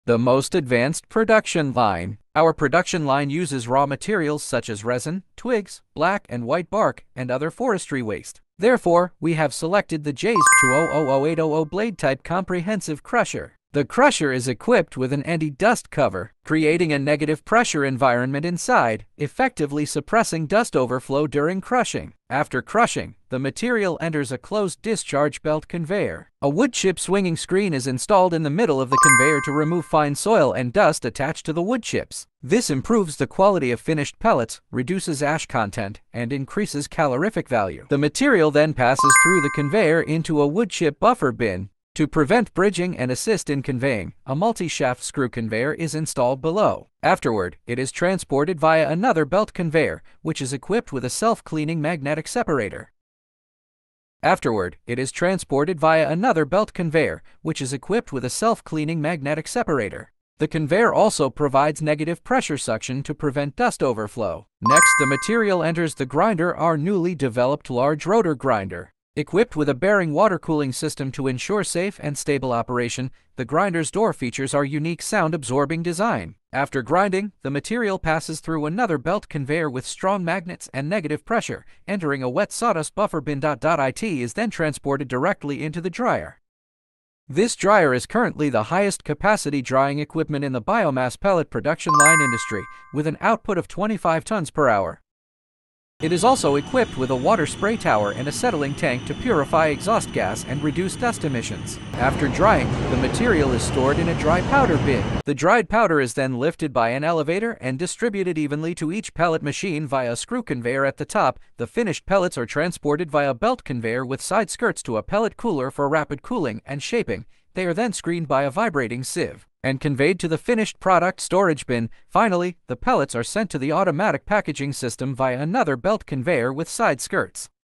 The Most Advanced Production Line
Our production line uses raw materials such as resin, twigs, black and white bark, and other forestry waste.
Therefore, we have selected the JZGHP2000-800 Blade-Type Comprehensive Crusher.
The crusher is equipped with an anti-dust cover, creating a negative pressure environment inside, effectively suppressing dust overflow during crushing.
After crushing, the material enters a closed discharge belt conveyor.
A wood chip swinging screen is installed in the middle of the conveyor to remove fine soil and dust attached to the wood chips.
This improves the quality of finished pellets, reduces ash content, and increases calorific value.
The material then passes through the conveyor into a wood chip buffer bin.
To prevent bridging and assist in conveying, a multi-shaft screw conveyor is installed below.
Afterward, it is transported via another belt conveyor, which is equipped with a self-cleaning magnetic separator.

The conveyor also provides negative pressure suction to prevent dust overflow.
Next, the material enters the grinder—our newly developed large-rotor grinder,

equipped with a bearing water-cooling system to ensure safe and stable operation.
The grinder’s door features our unique sound-absorbing design.
After grinding, the material passes through another belt conveyor with strong magnets and negative pressure,
entering a wet sawdust buffer bin.
It is then transported directly into the dryer.
This dryer is currently the highest-capacity drying equipment in the biomass pellet production line industry, with an output of 25 tons per hour.
It is also equipped with a water spray tower and a settling tank to purify exhaust gas and reduce dust emissions.
After drying, the material is stored in a dry powder bin.
Its structure features a live-bottom hydraulic bin, effectively preventing material bridging, ensuring smooth operation, reducing failure rates, and adding pressure relief and explosion-proof functions.
The dried powder is then lifted by an elevator and distributed evenly to each pellet machine via a screw conveyor at the top.
The finished pellets are transported via a belt conveyor with side skirts to a pellet cooler for rapid cooling and shaping.
They are then screened by a vibrating sieve and conveyed to the finished product storage bin.
Finally, the pellets are sent to the automatic packaging system via another belt conveyor with side skirts.